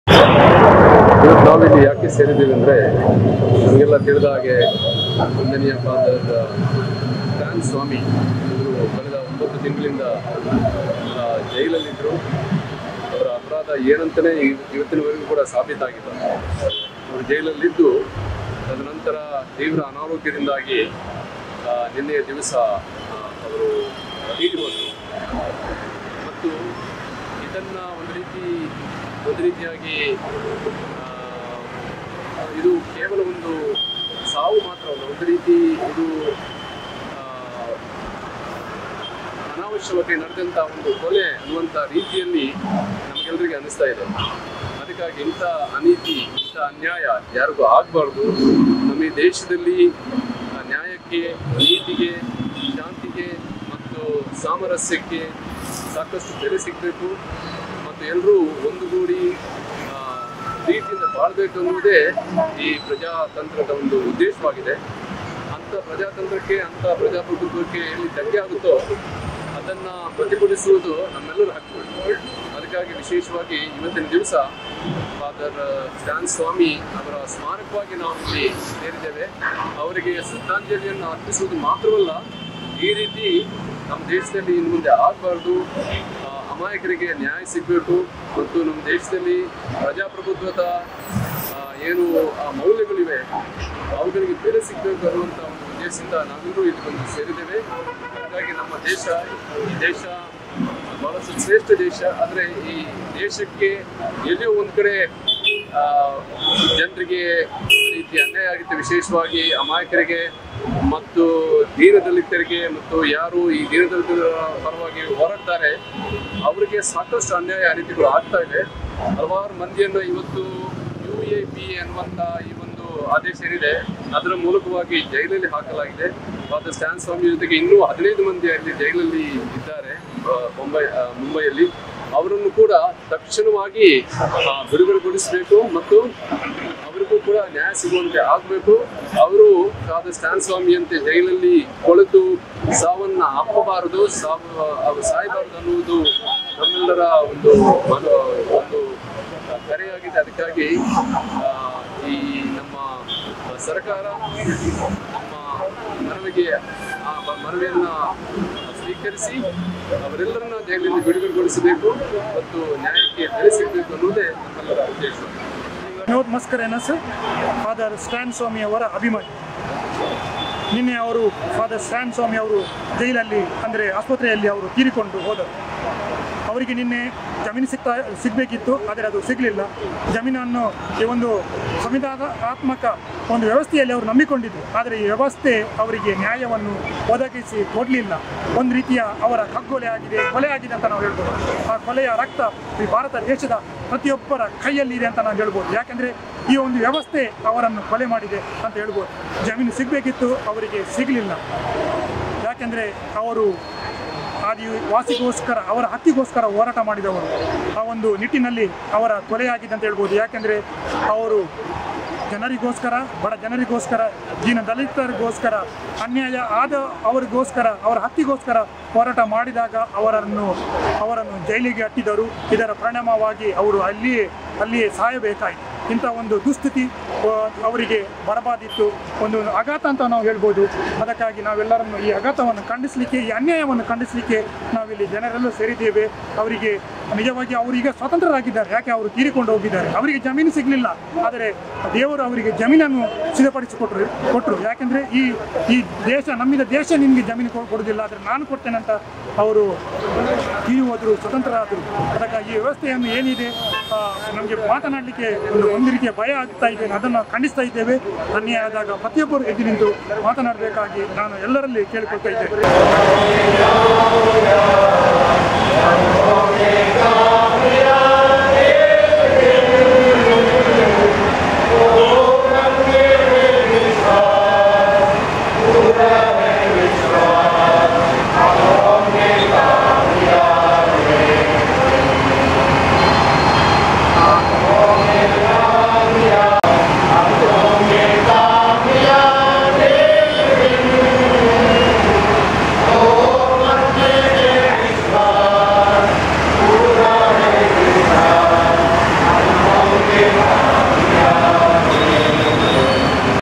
"ವ್ಯವಸ್ಥೆಯು ಸ್ಟ್ಯಾನ್ ಸ್ವಾಮಿಯವರಿಗೆ ನ್ಯಾಯ ಒದಗಿಸಲಿಲ್ಲ" | Stan Swamy
"ಸ್ಟ್ಯಾನ್ ಸ್ವಾಮಿಯವರ ಕಗ್ಗೊಲೆಯ ರಕ್ತ ಪ್ರತಿಯೊಬ್ಬ ಭಾರತೀಯರ ಕೈಯಲ್ಲಿದೆ "

"ಇಂತಹ ಅನ್ಯಾಯ ಯಾರಿಗೂ ಆಗಬಾರದು"

"ಕೋರ್ಟ್ ಜಾಮೀನು ನೀಡಿಲ್ಲ, ಈಗ ಸ್ಟ್ಯಾನ್ ಸ್ವಾಮಿ ಸ್ವತಂತ್ರರಾಗಿದ್ದಾರೆ"

ಮಂಗಳೂರಿನಲ್ಲಿ ಸಾಮಾಜಿಕ ಹೋರಾಟಗಾರ ಸ್ಟ್ಯಾನ್ ಸ್ವಾಮಿಯವರಿಗೆ ಶ್ರದ್ಧಾಂಜಲಿ